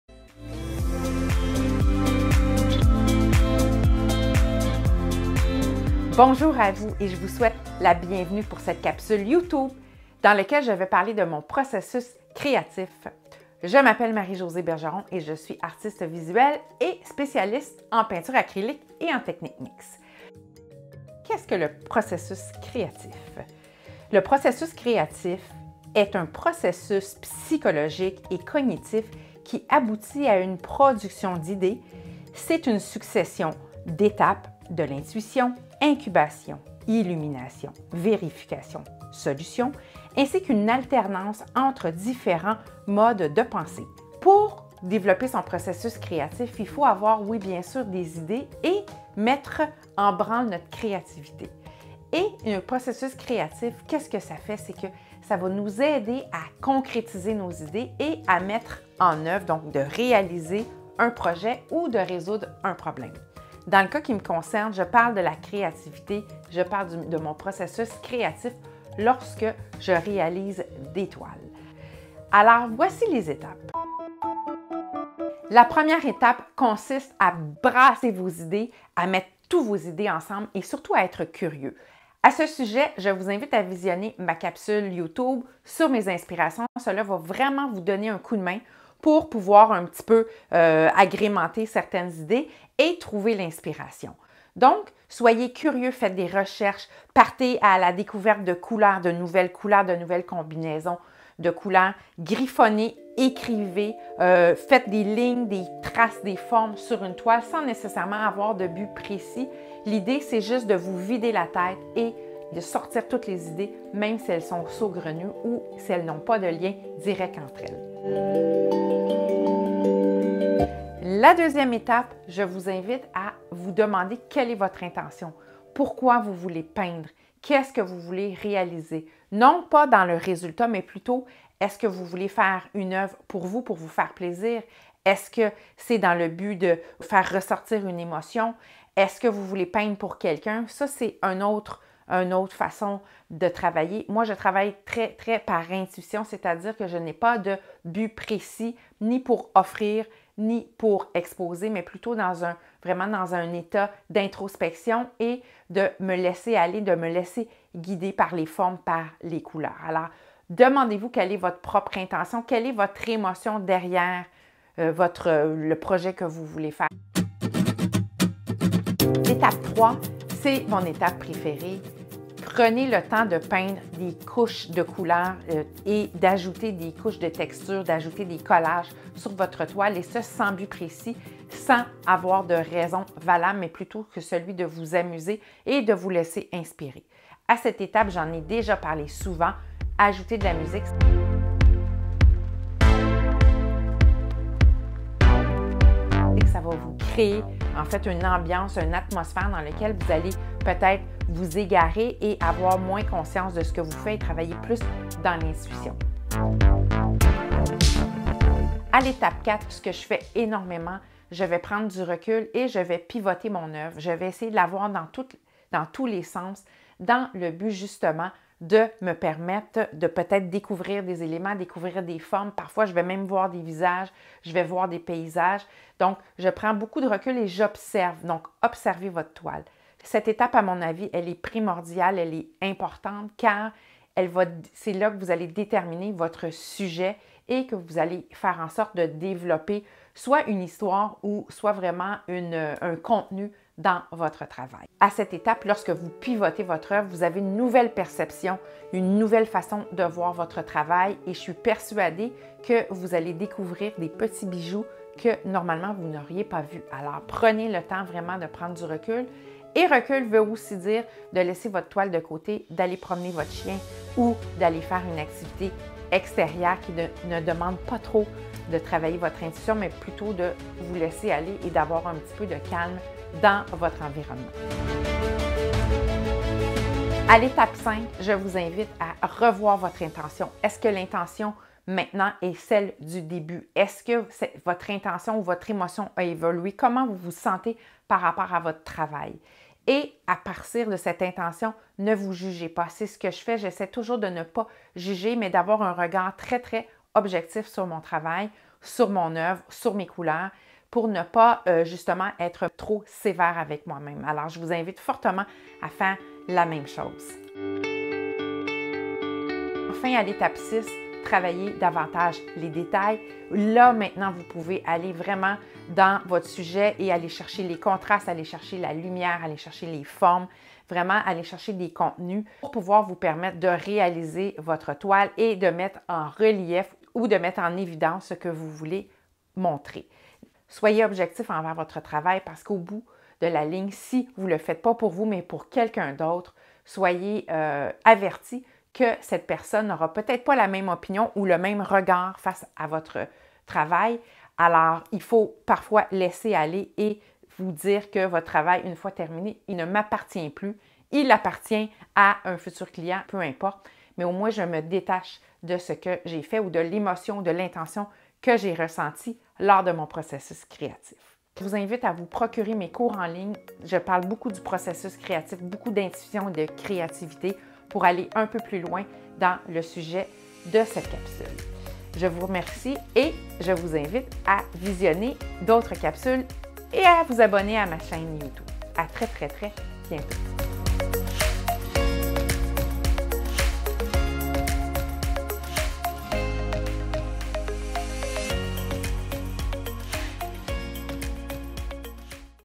Secrets de Marie-Josée, je vous partage mon processus créatif...(DU JAMAIS VUE!)
🎨 DÉCOUVREZ MES COURS EN LIGNE 🎨

Il me fait plaisir de vous parler de processus créatif en peinture acrylique et technique mixte. Dans cette capsule, je vous explique mon processus personnel que j'adopte lorsque je débute un nouveau projet artistique. En 6 étapes faciles à suivre, je vous invite à vous initier à cette méthode.

Chapitres
0:00 - Intro
0:20 - Le processus créatif expliqué
1:13 - Étape 1: Le brassage d'idées
1:58 - Étape 2: L'intention
2:47 - Étape 3: Jouer avec la toile et les couleurs (phase illumination)
3:27 - Étape 4: Le recul et la perception
4:36 - Étape 5: Revenir sur l'intention initiale
5:08 - Étape 6: La concrétisation de l'idée et les détails
5:31 - Le détachement
6:02 - Mot de la fin